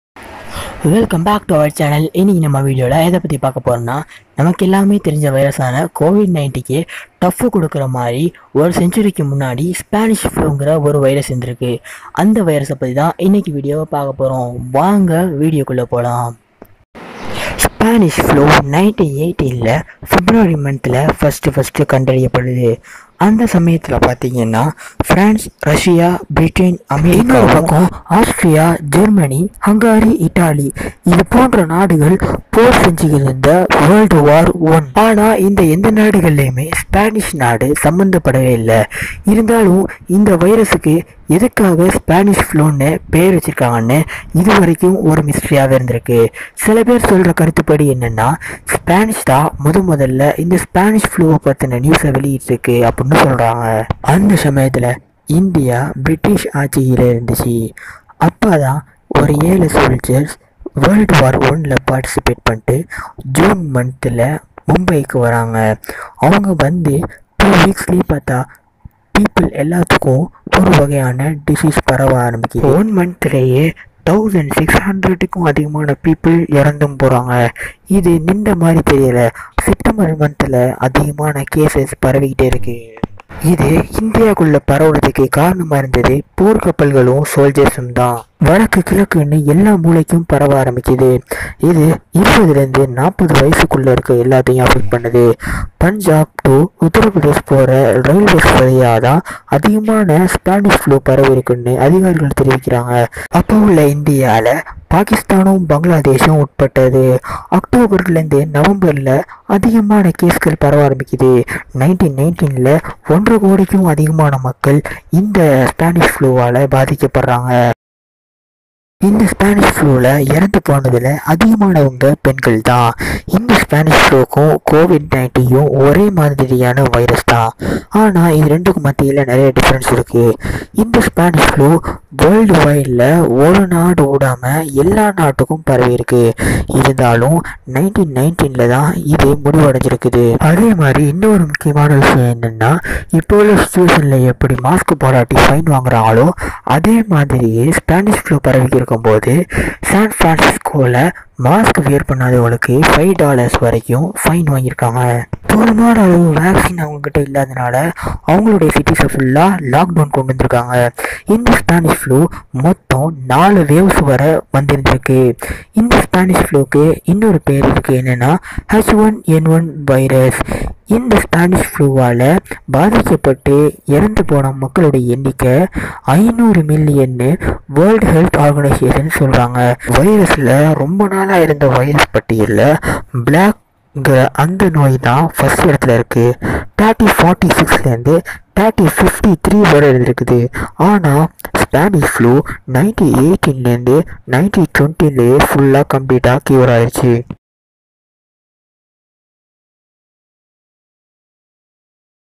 Covid 19 கே பயத்தை காட்டிய virus | spanish flu explained | தமிழ் |Spartan tamizha
Be safe. Be secure. Wear mask. Follow rules.

☣️Keep supporting to our channel. 🔥

🎯On process for next video and keep  ⚔️
⚙️. connected. ⚙️

📲. Check out over old videos. 📲

H5N virus

covid-19vaccine